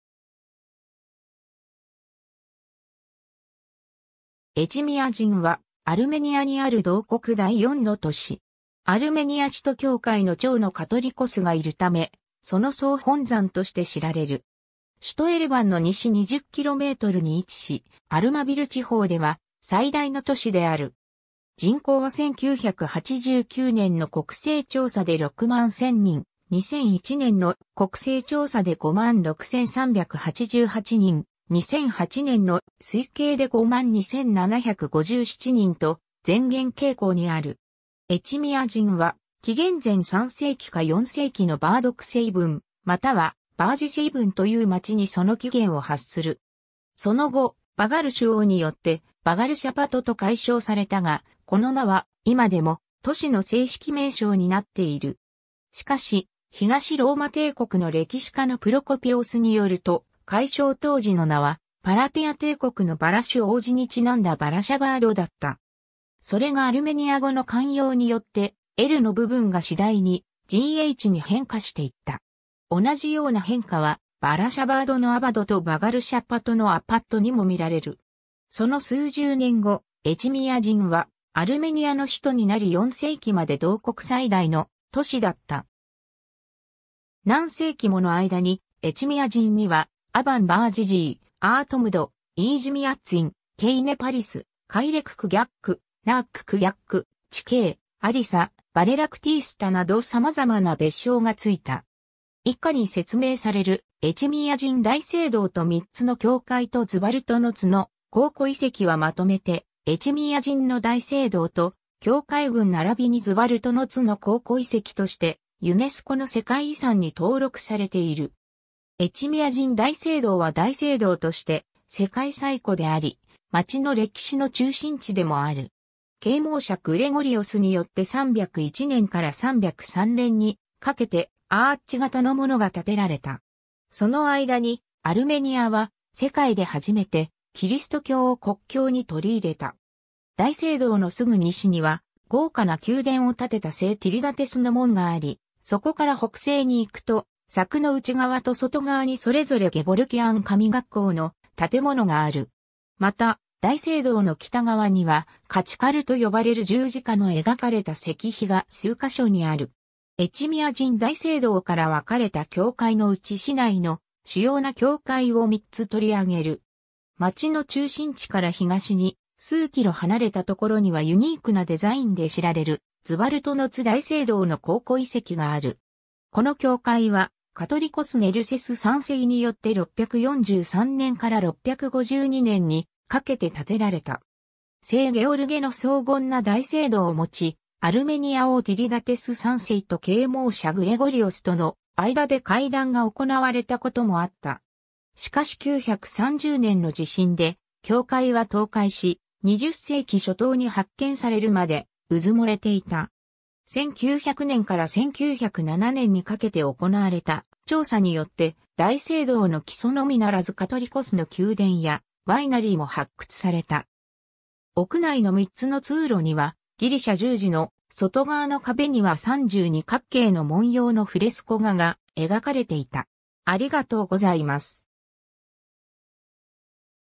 エチミアジン
エチミアジン（アルメニア語: Էջմիածին、ラテン文字転写：Ejmiatsin、Echmiatsin、Etchmiadzin、Echmiadzin）は、アルメニアにある同国第四の都市。
アルメニア使徒教会の長のカトリコスが居るため、その総本山として知られる。
首都エレバンの西20kmに位置し、アルマヴィル地方では最大の都市である。
人口は1989年の国勢調査で61,000人、2001年の国勢調査で56,388人、2008年の推計で52,757人と漸減傾向にある。
 エチミアジンは紀元前3世紀か4世紀のVardkesavanまたはVardgesavanという街にその起源を発する。
その後、ヴァガルシュ王（117年 - 140年）によってヴァガルシャパト（Վաղարշապատ、Vagharshapat）と改称されたが、この名は今でも都市の正式名称になっている。
しかし、東ローマ帝国の歴史家のプロコピオスによると改称当時の名はパラティア帝国のヴァラシュ王子にちなんだヴァラシャバード（Valashabad、ヴァラシュの街の意）だった。
それがアルメニア語の慣用によってLの部分が次第にGhに変化していった。
同じような変化はヴァラシャバードのabadとヴァガルシャパトのapatにも見られる。
 その数十年後、エチミアジンはアルメニアの首都になり4世紀まで同国最大の都市だった。
 何世紀もの間に、エチミアジンにはAvan Vardgesi、Artemed、Iejmiatsin、Kaynepolis、Kayrak’aghak’、 Norak’aghak’、Uch’k’ilisa、Valeroktistaなど様々な別称がついた。
 以下に説明されるエチミアジン大聖堂と3つの教会とズヴァルトノツの考古遺跡はまとめてエチミアジンの大聖堂と教会群ならびにズヴァルトノツの考古遺跡としてユネスコの世界遺産に登録されている。
 エチミアジン大聖堂は大聖堂（主教座聖堂）として世界最古であり、街の歴史の中心地でもある。
啓蒙者グレゴリオスによって301年から303年にかけてアーチ型のものが建てられた。
その間に、アルメニアは世界で初めてキリスト教を国教に取り入れた。
 大聖堂のすぐ西には豪華な宮殿を建てた聖ティリダテスの門があり、そこから北西に行くと柵の内側と外側にそれぞれゲヴォルキアン神学校の建物がある。
また、大聖堂の北側にはカチカルと呼ばれる十字架の描かれた石碑が数箇所にある。
 エチミアジン大聖堂から分かれた教会のうち市内の主要な教会を3つ取り上げる。
 街の中心地から東に数キロ離れたところにはユニークなデザインで知られるズヴァルトノツ大聖堂の考古遺跡がある。
この教会はカトリコス・ネルセス3世（建設者の異名を持つ）によって643年から652年にかけて建てられた。
聖ゲオルゲの荘厳な大聖堂を持ち、アルメニア王ティリダテス3世と啓蒙者グレゴリオスとの間で会談が行われたこともあった。
 しかし930年の地震で教会は倒壊し、20世紀初頭に発見されるまで埋もれていた。
1900年から1907年にかけて行われた調査によって、大聖堂の基礎のみならずカトリコスの宮殿やワイナリーも発掘された。
屋内の3つの通路にはギリシャ十字の、外側の壁には32角形（ほぼ円）の文様のフレスコ画が描かれていた。